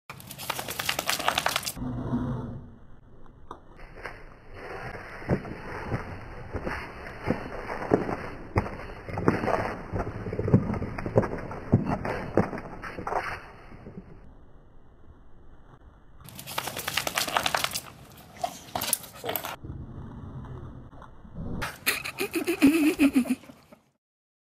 Hoshi shake analyzed ホシのぶるぶるを分析する
A VERY scientific look at a dog shake.  I'm milking this 2-second clip for all its worth, don't forget to watch "Hoshi brain shake!" too.

犬のぶるぶるを科学的に分析。　こんなネタでひっぱります、「ホシのぶるぶる」もよろしく。